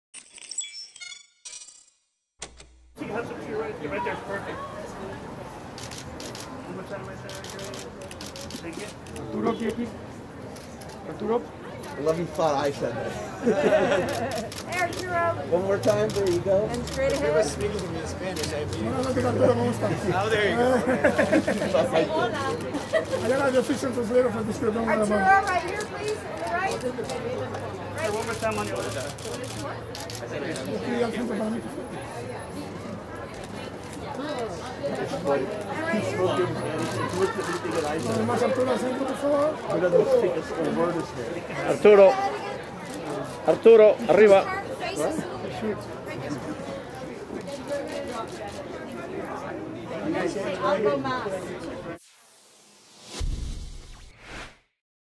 Arturo Castro "Silent Night" Los Angeles Special Screening Red Carpet
Broll footage: Guatemalan actor Arturo Castro on the red carpet at the special screening of AMC+/RLJE Films' darkly comedic drama/horror film "Silent Night" held at Neuehouse Hollywood in Los Angeles, California USA on November 30, 2021